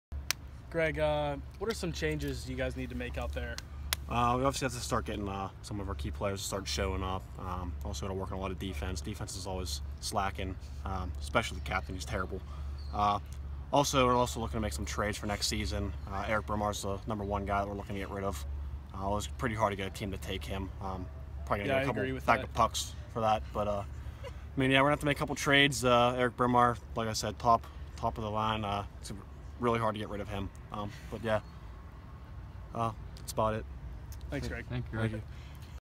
BOOZERS vs Shaler POSTGAME: Greg Guckert
UPMC Lemieux Sports Complex- Boozers Captain Greg Guckert answers questions following tie with Shaler HS.